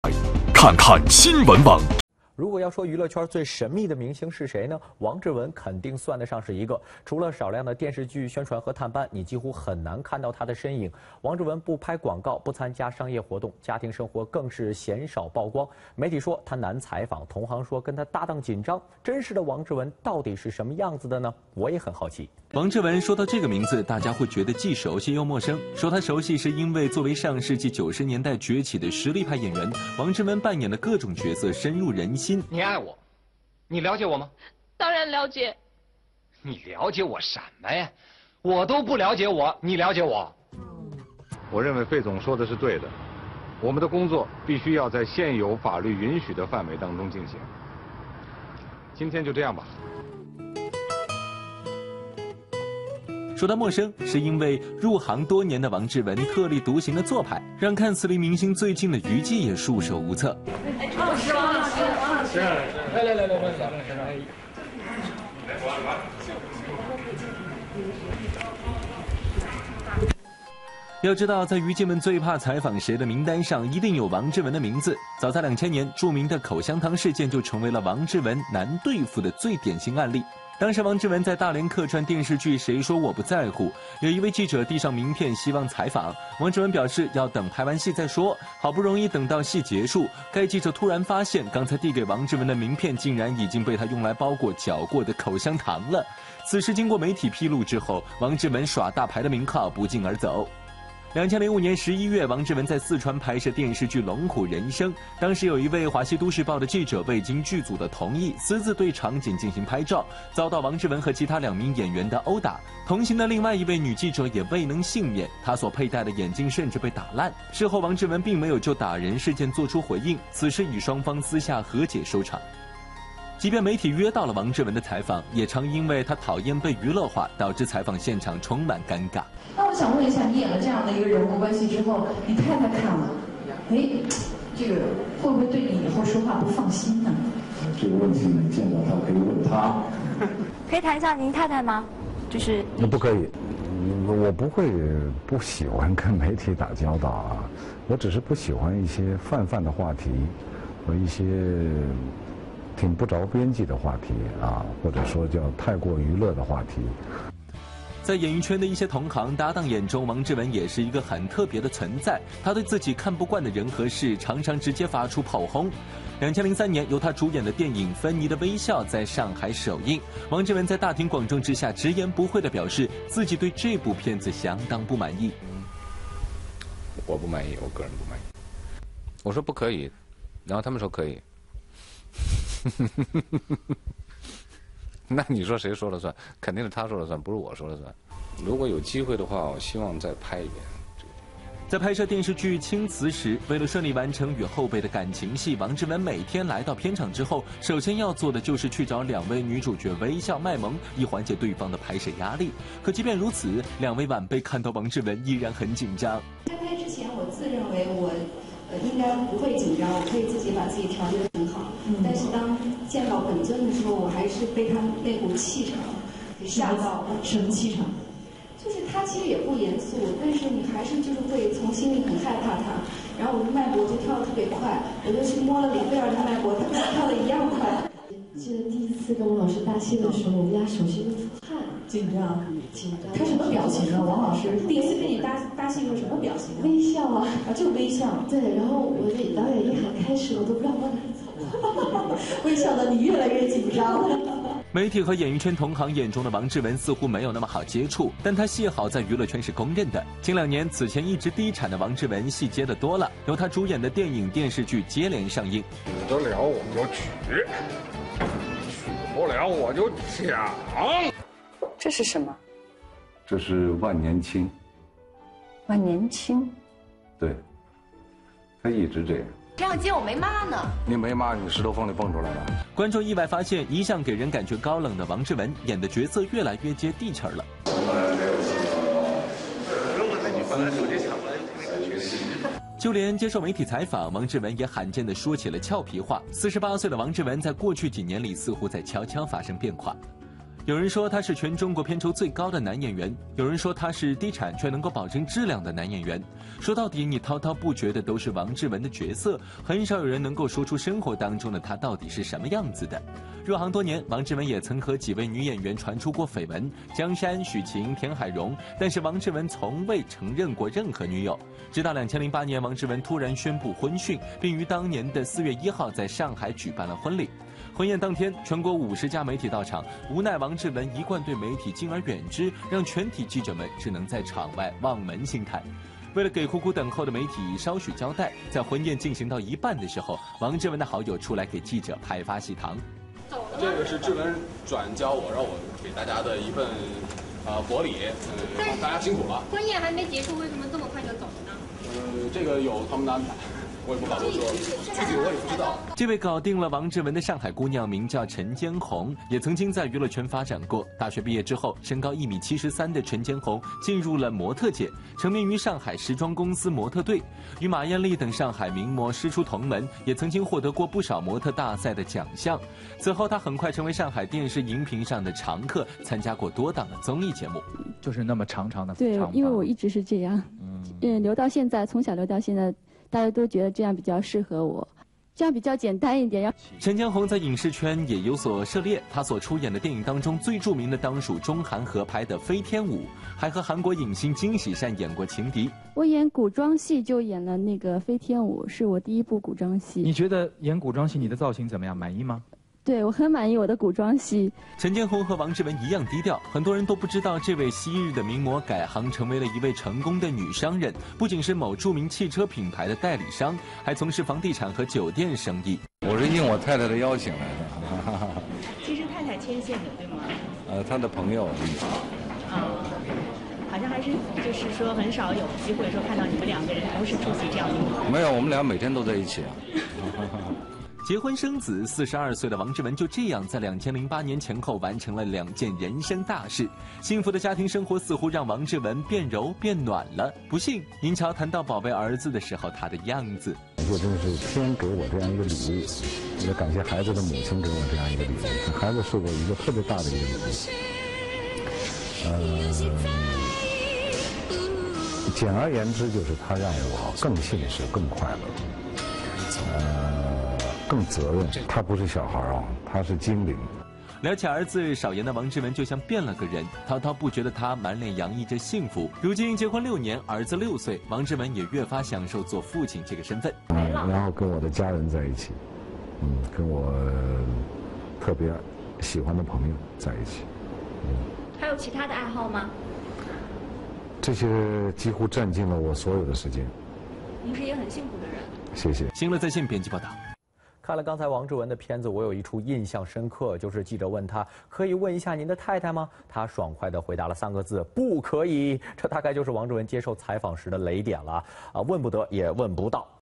王志文：戏里百变 戏外神秘
正在北京拍摄的都市情感电视轻喜剧《待嫁老爸》中，王志文饰演一位“待嫁老爸”，不仅要单身抚养处于叛逆期的女儿，还要同时面对朱丹、陈好、刘蓓饰演的三个不同类型女人的“情感夹击”。